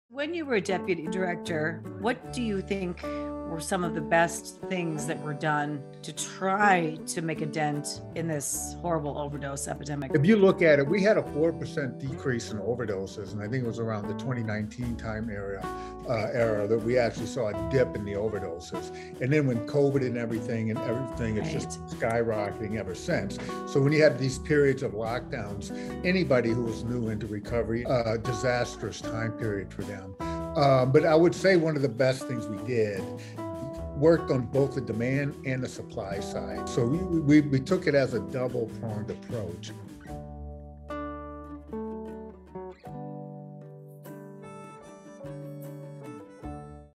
Episode 89 | Former U.S. Deputy Drug Czar on ending overdose crisis
Art Kleinschmidt was the Deputy Director of the White House Office of National Drug Control Policy or Deputy Drug Czar under President Trump. He also served on the Domestic Policy Council in the White House as Senior Advisor for mental health & addiction, as well as the same role for SAMHSA.

Art is in long-term recovery from substance use disorder and became a licensed clinician with extensive treatment experience. He is also the founder of the Recovery for America Now Foundation, which provides treatment scholarships to treatment centers across the country. Art discusses his unconventional path to D.C. and how to solve the overdose crisis in this episode of Grieving Out Loud.